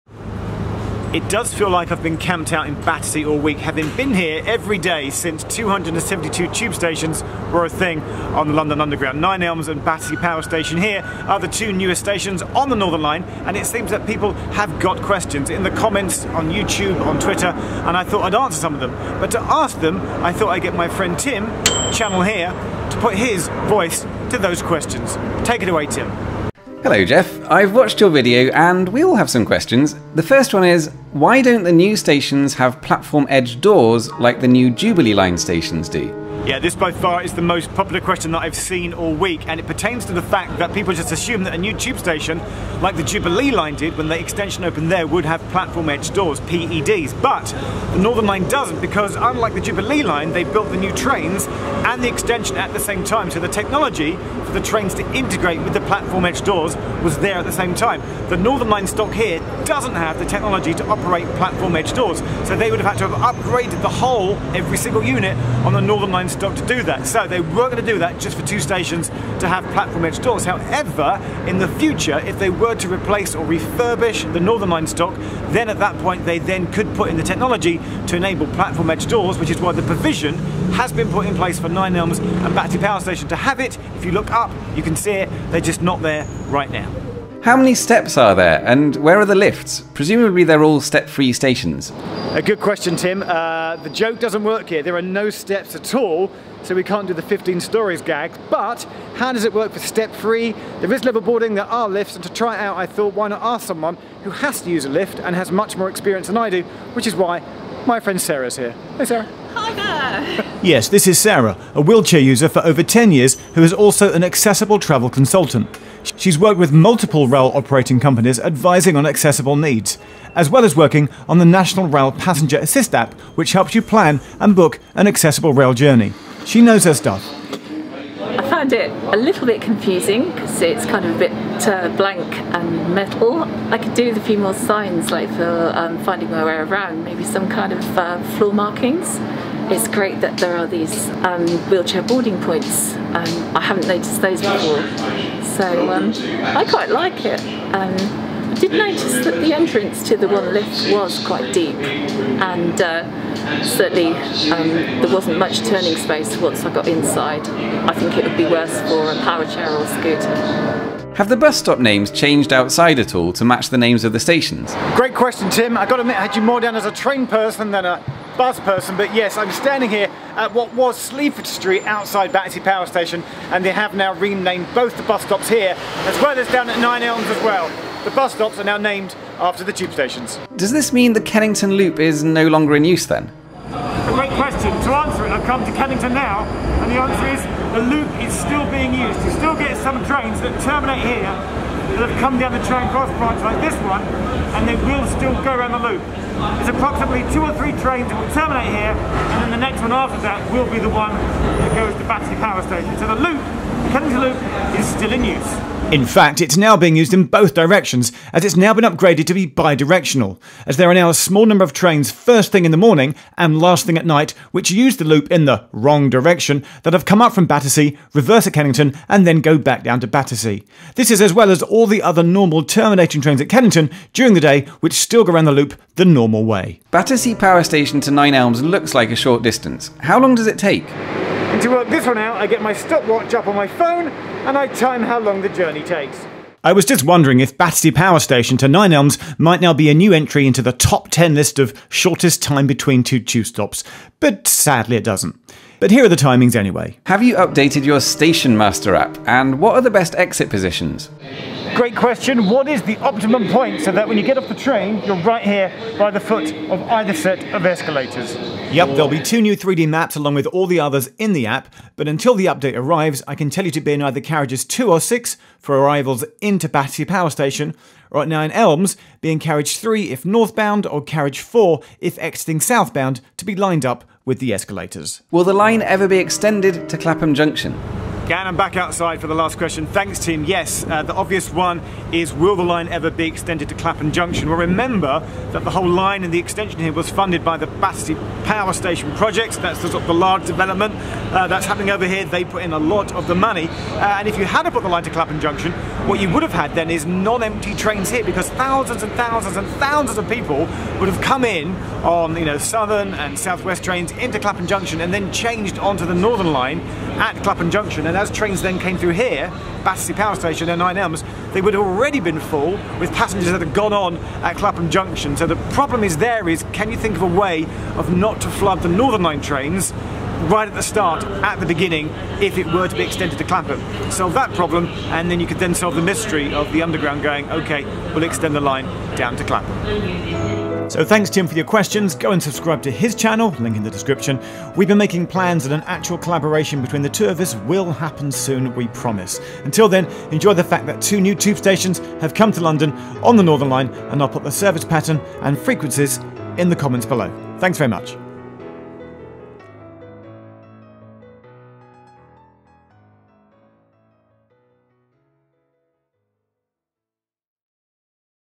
Battersea Tube Extension - Your Questions Answered
So we have 272 Tube Stations Stations on the London Underground now, and after last weeks fanfare of a launch, it seemed people had a few questions, let's see if can clear some of them up ...

Download the Station Master App (iOs only) with 3D maps and exit positions for the entire Underground and more,

OSI's
Finally, there is NO Out-of-Station interchange between Nine Elms and Vauxhall even though it's a short distance, totally walkable and I think there should be.  The OSI's for the new extension that *do* exist are just between Battersea Power Station (Tube) and Battersea park (National Rail) and Queenstown Road (National Rail).